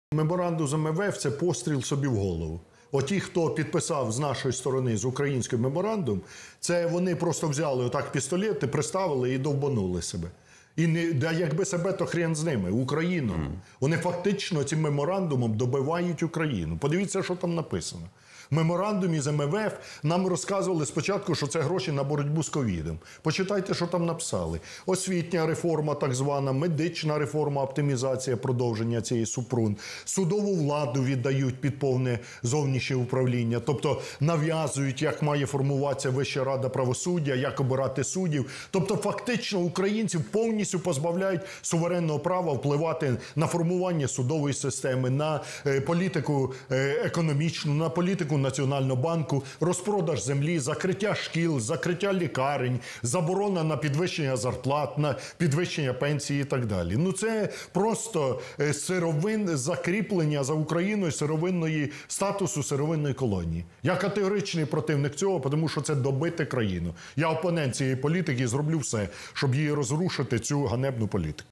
Ляшко: Меморандум з МВФ – це постріл собі в голову
Залежність від позик Міжнародного валютного фонду добиває Україну. У цьому переконаний Олег Ляшко. В ефірі телеканалу «Україна 24» політик розповів, чим загрожує країні новий Меморандум з МВФ і як злізти з голки міжнародних кредитів.
Та якби ж себе, то хрен з ними – Україну довбонули. Нам розказували спочатку, що ці гроші – на боротьбу з COVID. А почитайте, що там написано: продовження освітньої, медичної, судової реформ, закриття шкіл, лікарень, розпродаж землі, заборона на підвищення зарплат і пенсій, позбавлення суверенного права українців на формування судової системи, економічної політики. Тобто, це закріплення за Україною статусу сировинної колонії», - вважає Ляшко.
Лідер РПЛ наголошує: гроші МВФ мають піти не на покриття дірок у бюджеті, а на розвиток власного виробництва і створення нових робочих місць. Тоді Україна зможе відмовитись від міжнародних позик, вважає Олег Ляшко.
«Я категоричний противник політики, яку нам нав’язує МВФ, бо це добиває Україну. Я її опонент і зроблю все, щоб її поміняти», - підсумував Ляшко.